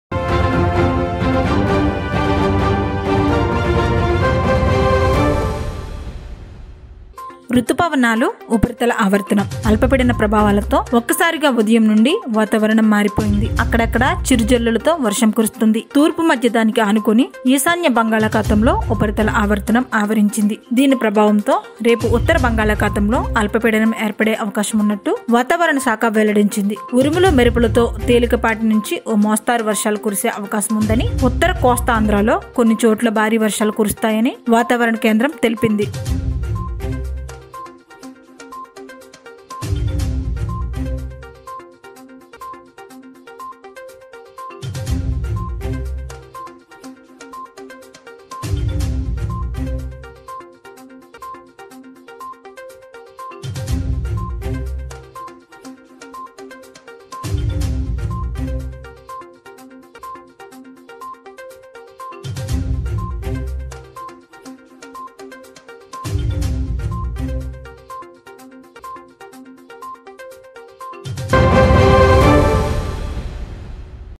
ఆంధ్రప్రదేశ్:అల్పపీడన ప్రభావాలతో కొన్ని చోట్ల భారీ వర్షాలు.-వాతావరణ శాఖ
ఆంధ్రప్రదేశ్:అల్పపీడన ప్రభావాలతో ఒక్కసారిగా ఉదయం నుండి వాతావరణం మారిపోయింది.ఉత్తర బంగాళాఖాతంలో అల్పపీడనం ఏర్పడే అవకాశం-వాతావరణ శాఖ